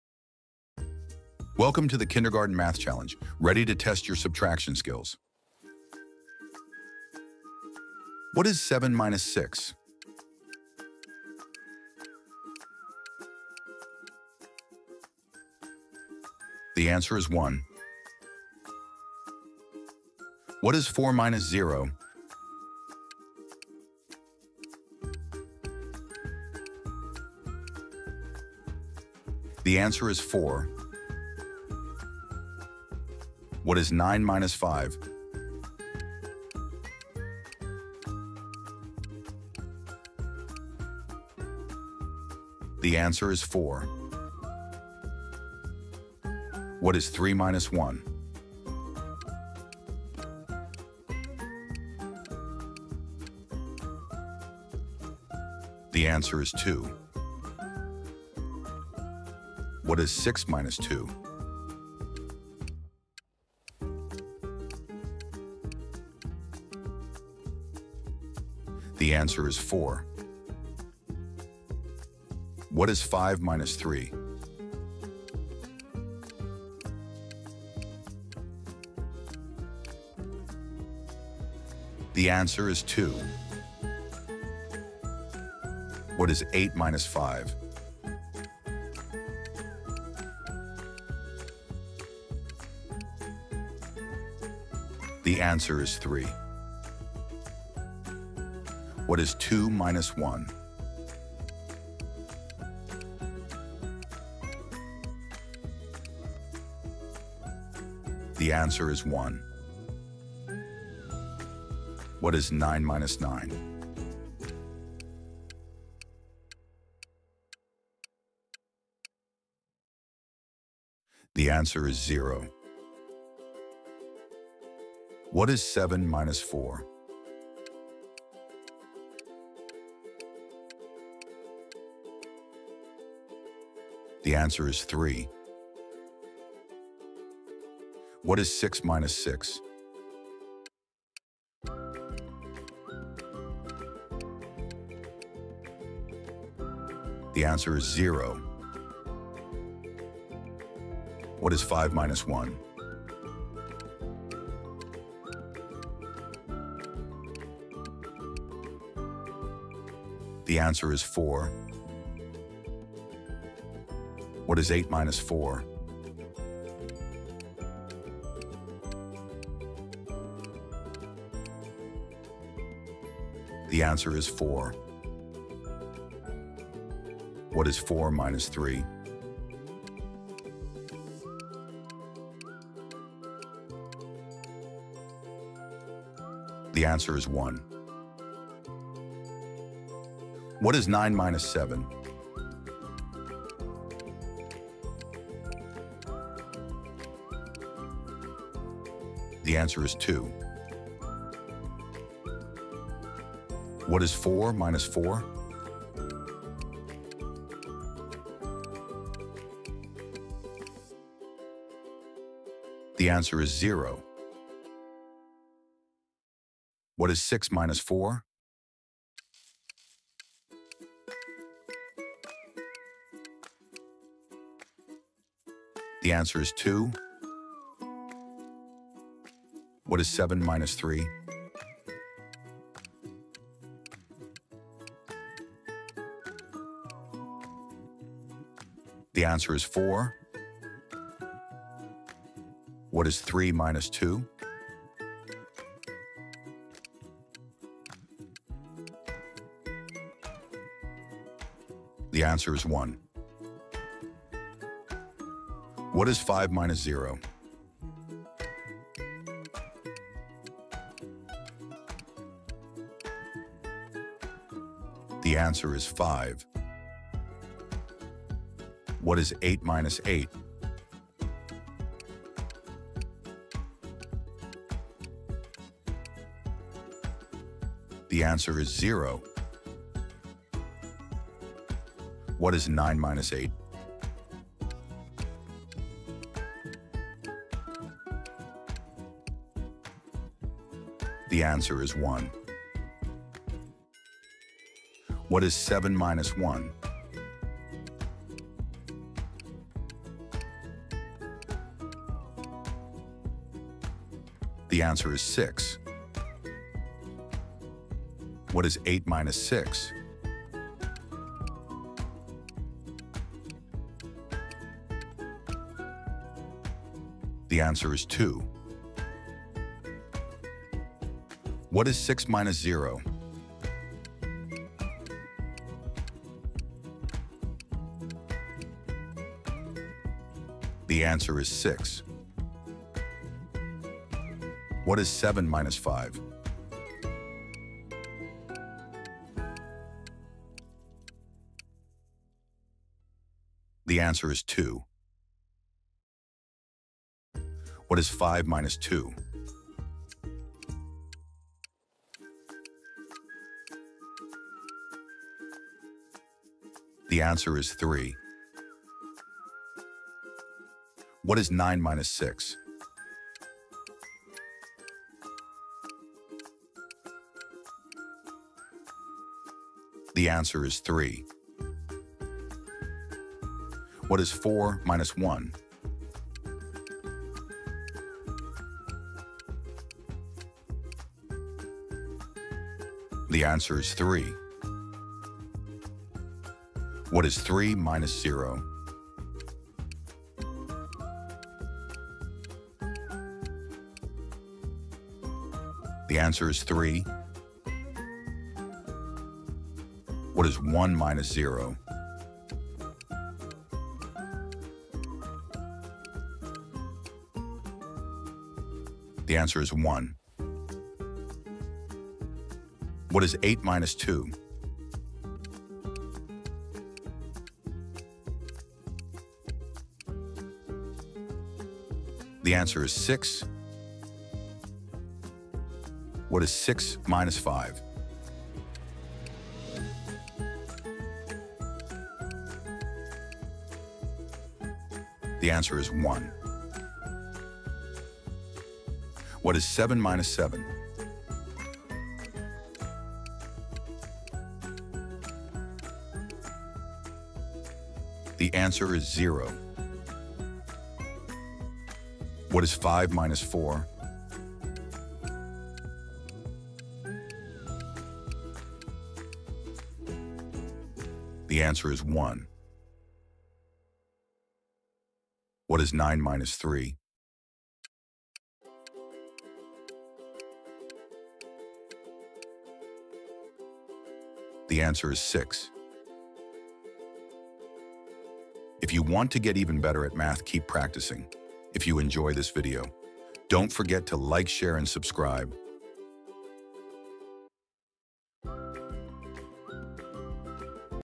Kindergarten Math Practice | Subtraction Quiz for Kids 🎯 | Easy Take Away Learning
Learn how to take away numbers with this simple and fun Subtraction quiz for kids! 🌟
This video is perfect for Kindergarten learners who are beginning to understand subtraction. With bright visuals, easy questions, and interactive activities, children will enjoy learning math while building early number skills.

🧮 In this Subtraction quiz for kids, children will:
✔️ Practice subtracting numbers from 0 to 10
✔️ Learn the concept of taking away
✔️ Improve counting and number recognition
✔️ Build confidence through playful math practice

🎓 Perfect for:

Kindergarten and preschool students

Homeschool and classroom lessons

Parents and teachers looking for engaging math content

🎯 Watch, pause, and play along to master subtraction in a fun way!